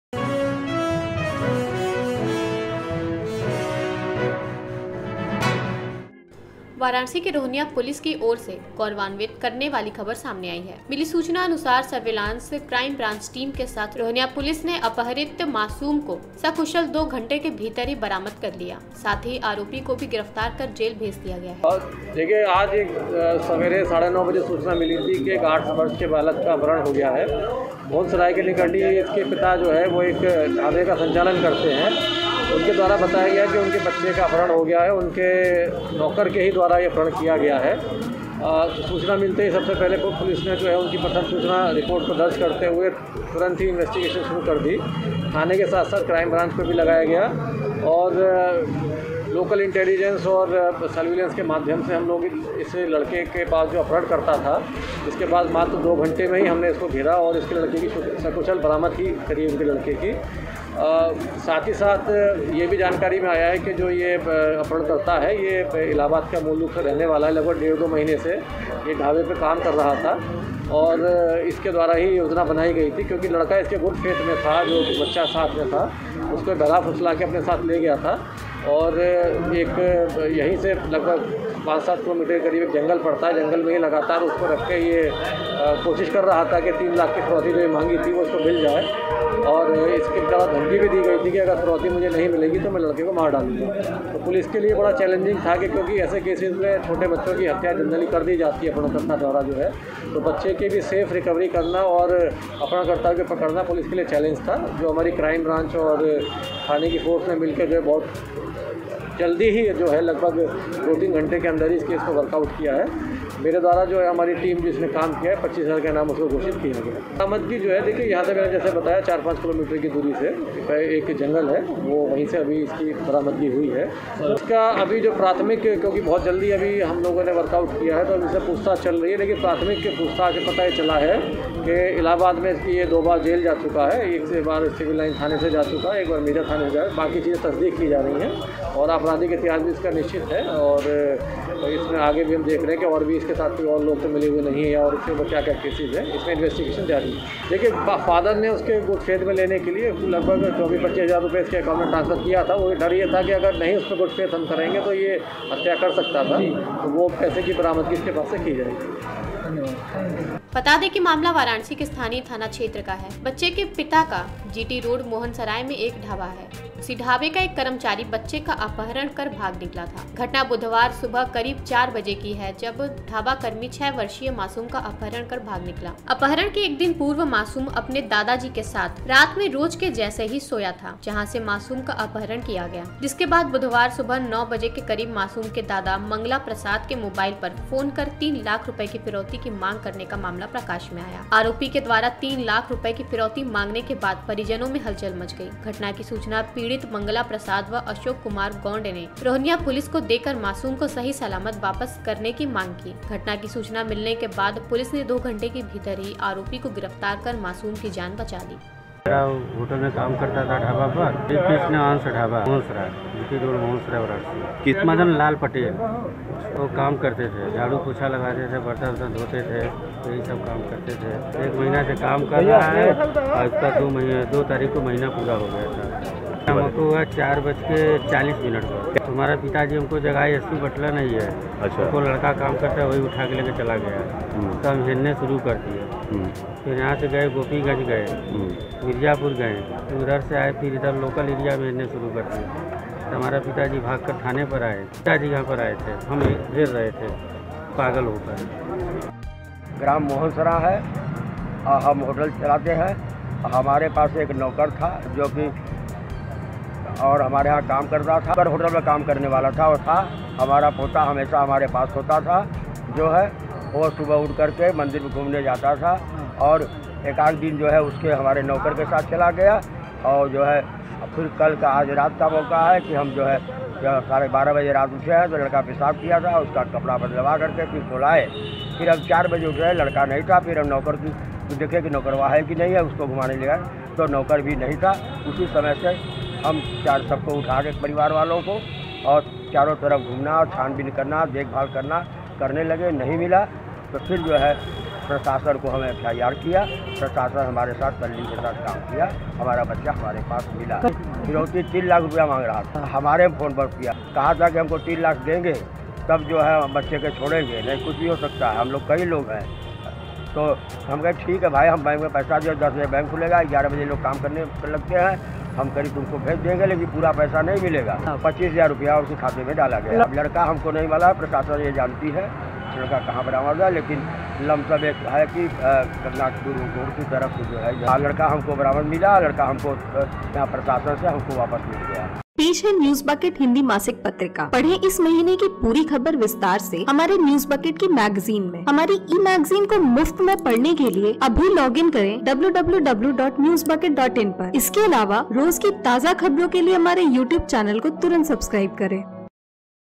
Varanasi में हुआ था 6 साल के मासूम का अपहरण, Police ने दो घंटों के भीतर ही आरोपी को किया Arrest
वाराणसी के रोहनिया पुलिस की ओर गौरवान्वित करने वाली खबर सामने आयी है। मिली सूचना अनुसार सर्विलांस, क्राइम ब्रांच टीम के साथ रोहनिया पुलिस ने अपहृत मासूम को सकुशल दो घंटों के भीतर ही बरामद कर लिया। साथ ही आरोपी को भी गिरफ्तार कर जेल भेज दिया गया है। Watch the complete Video For More Details!

Gadgets We Use:

Green Chroma